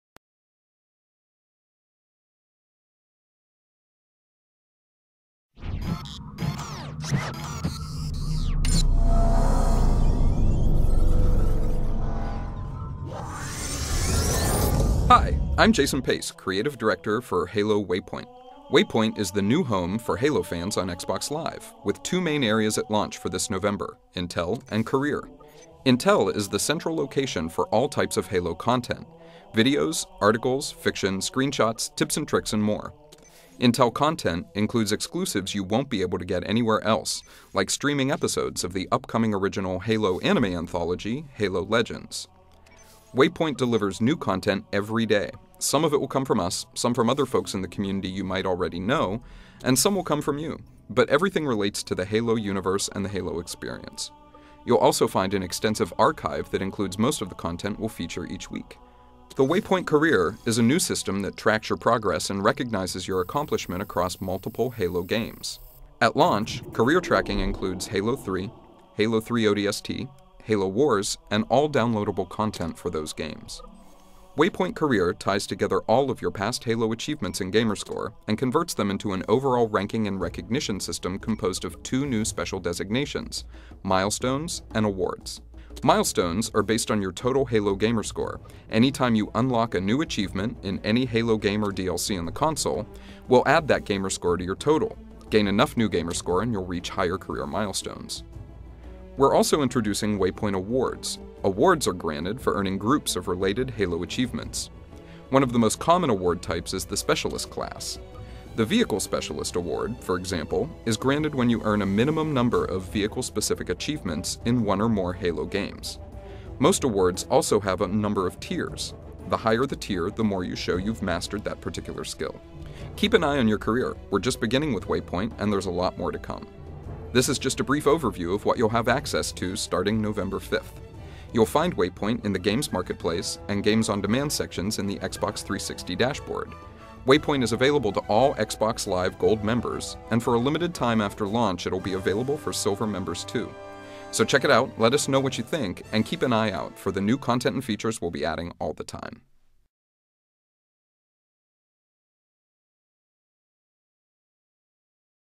Halo Waypoint: Developer Diary Walkthrough [HD]
Take a look at what Halo Waypoint will have to offer on Xbox Live.